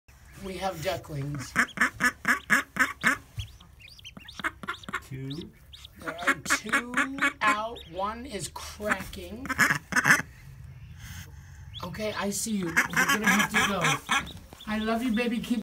Ducklings My First Natural Hatching.
My first Ducklings Hatching on the farm.